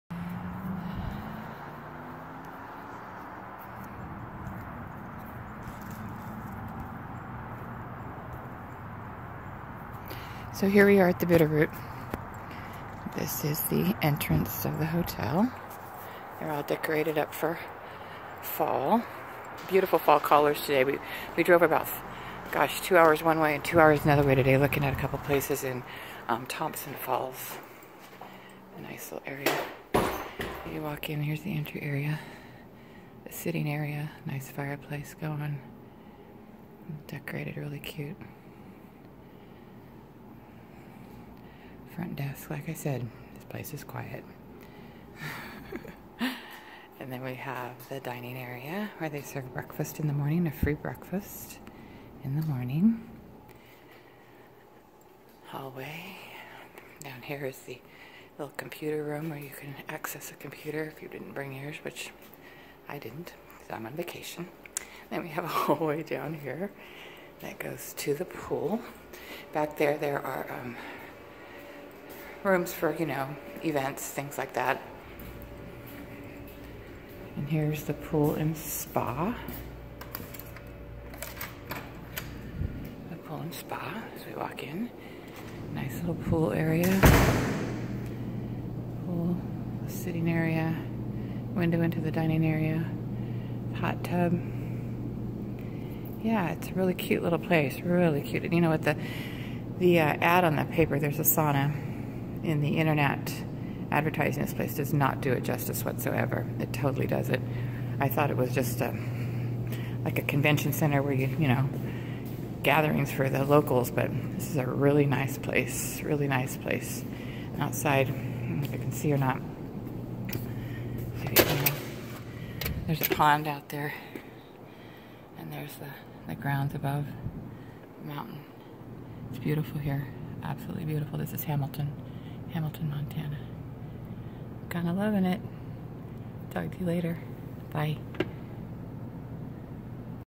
Paula, Saved 25% off her Vacation at the Bitterroot Inn in Montana
Paula, saved 25% off her vacation stay at the Bitterroot Inn in Montana. Paula, has had a lifelong dream of living in Montana but it was only recently after having success starting a home business that she decided to visit Montana to check out where she wanted to live. She used her travel package and saved 25% off her accommodations at the Bitterroot Inn and she also saved 20% off her car rental.